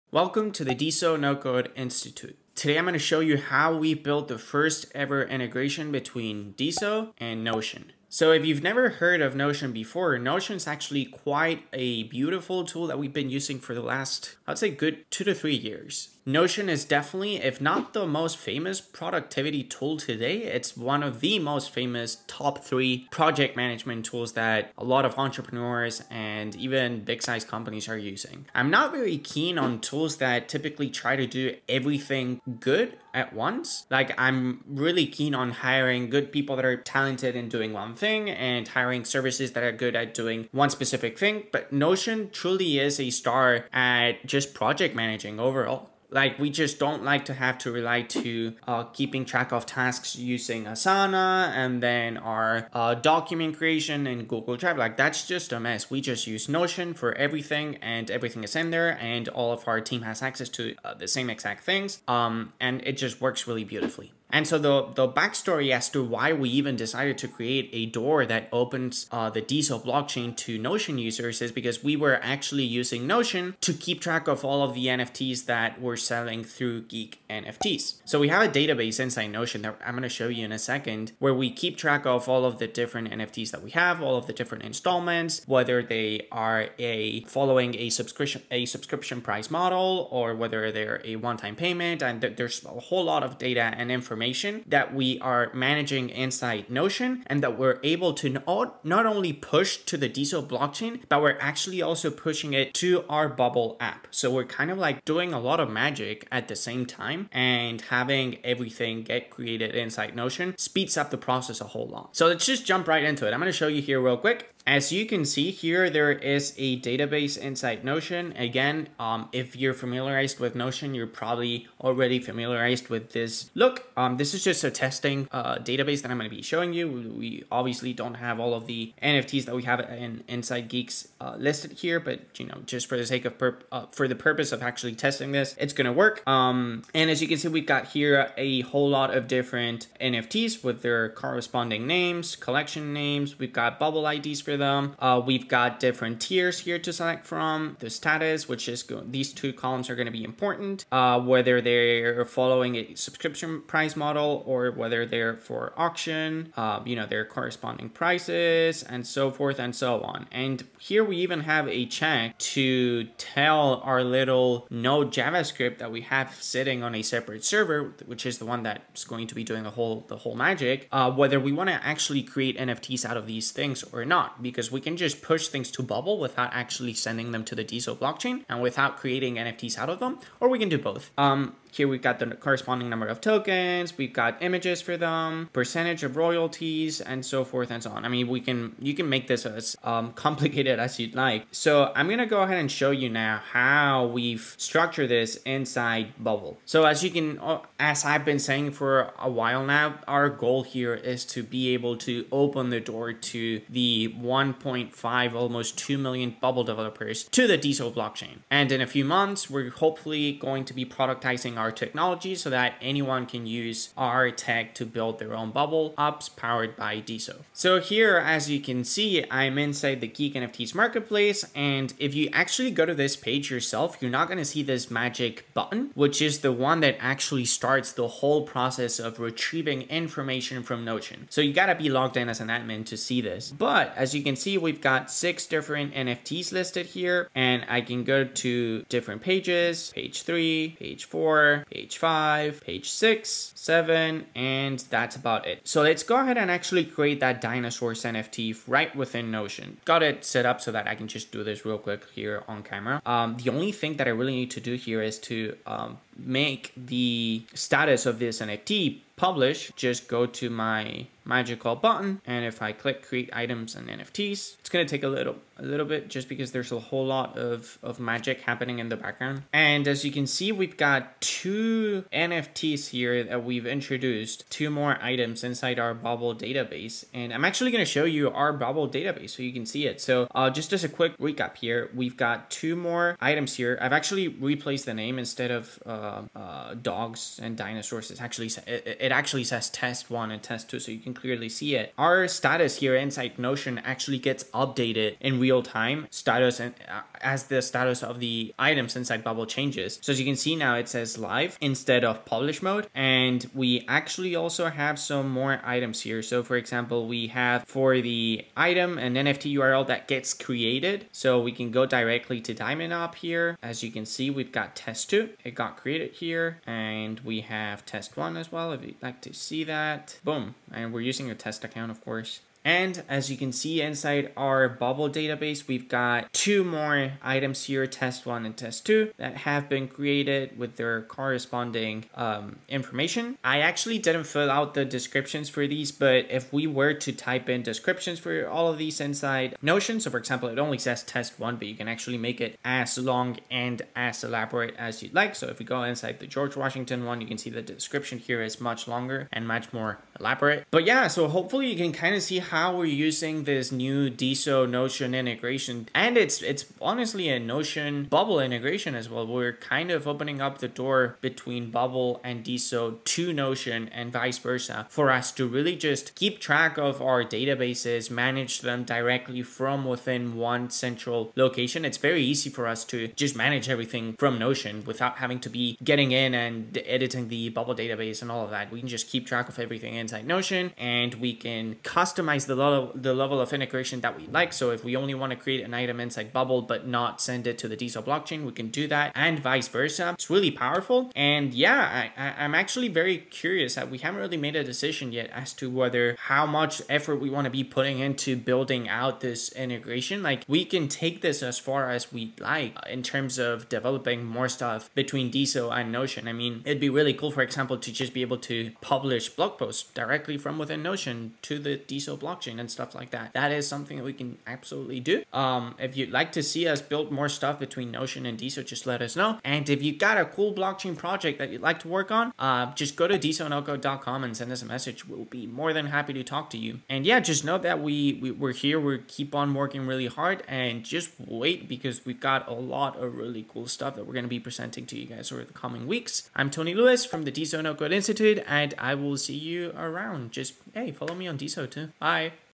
First ever integration between Notion.so and the Deso Blockchain
Watch @tonylewis break down how we've integrated notion.so with both @deso and bubble.io

We've been using Notion as our main productivity tool here at Deso No-code. We figured that since we were already using Notion to keep track of NFT items that we are selling through @geekNFTs it also made sense to build out an integration that would allow us to actually create NFTs directly from Notion.

And, well, we did just that.

We feed our bubble marketplace all of the data parameters from our Notion database and push them to the Deso blockchain too.